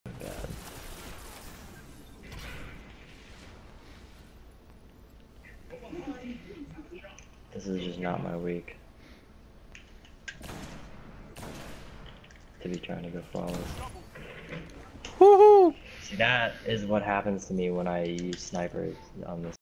Collateral Snipe
1 sniper shot 2 dead guardians. Woo woo